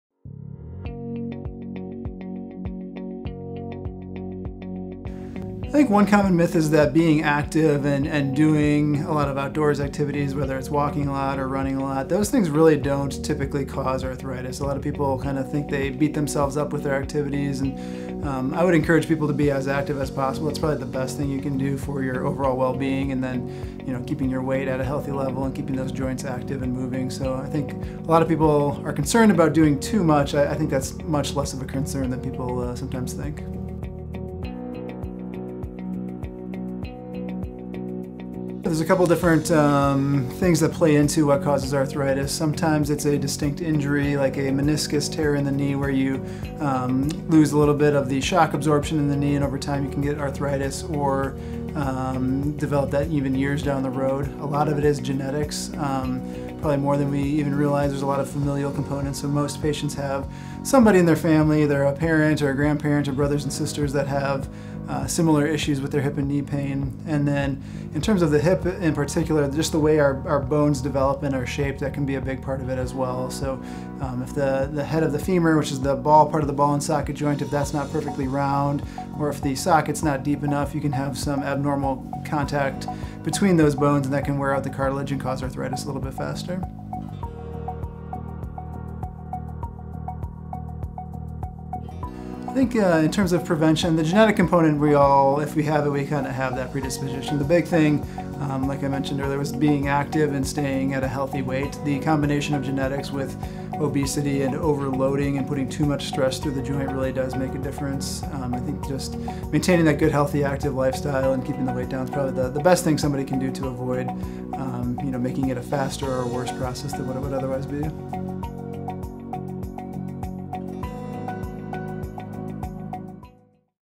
Common myths about arthritis - Mayo Clinic Health System
Marcus Greatens, M.D., an orthopedic surgeon at Mayo Clinic Health System discusses some common myths associated with arthritis, as well as a few of the more common causes of arthritis of the hip and knee.

Mayo Clinic Health System offers outstanding care close to home. Part of Mayo Clinic, we meet most of your health care needs locally.

Follow Mayo Clinic Health System on